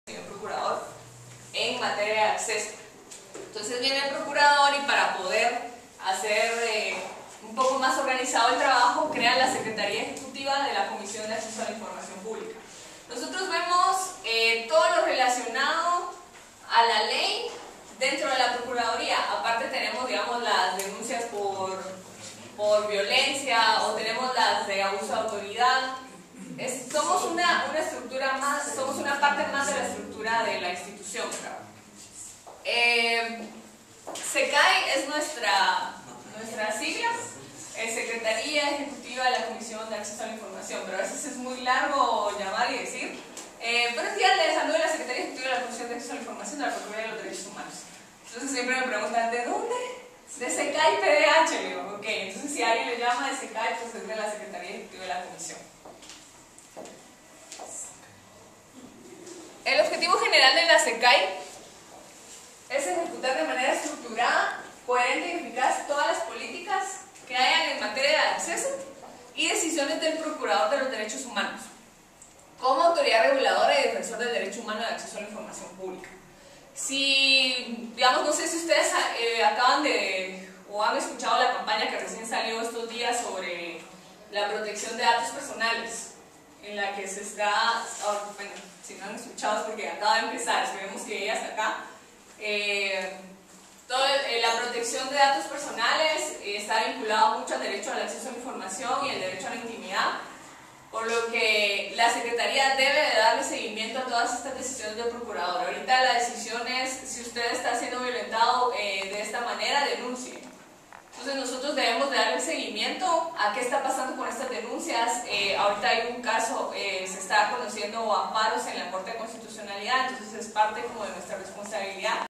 Participación de Silvia Angel de SECAI
Patzún, Participación de Silvia Angel durante el taller sobre la Ley de Acceso a la Información Pública de SECAI (Secretaría Ejecutiva de la Comisión de Acceso a la Información) de la PDH.